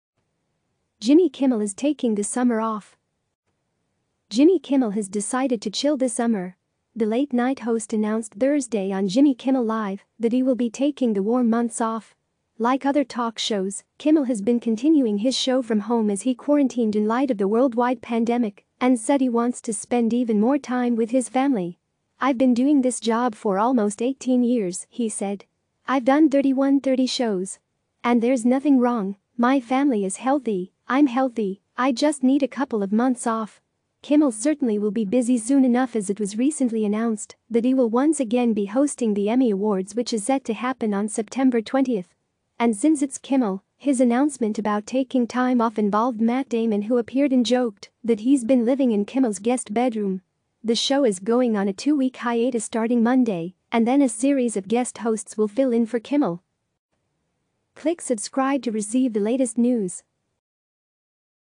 Jimmy Kimmel is taking the summer off
Jimmy Kimmel has decided to chill this summer.

The late night host announced Thursday on "Jimmy Kimmel Live" that he will be taking the warm months off.

Like other talk shows, Kimmel has been continuing his show from home as he quarantined in light of the worldwide pandemic and said he wants to spend even more time with his family.

"I've been doing this job for almost 18 years," he said. "I've done 3,130 shows. And there's nothing wrong, my family is healthy, I'm healthy, I just need a couple of months off."

Kimmel certainly will be busy soon enough as it was recently announced that he will once again be hosting the Emmy Awards which is set to happen on September 20.

And since it's Kimmel, his announcement about taking time off involved Matt Damon who appeared and joked that he's been living in Kimmel's guest bedroom.

The show is going on a two-week hiatus starting Monday and then a series of guest hosts will fill in for Kimmel.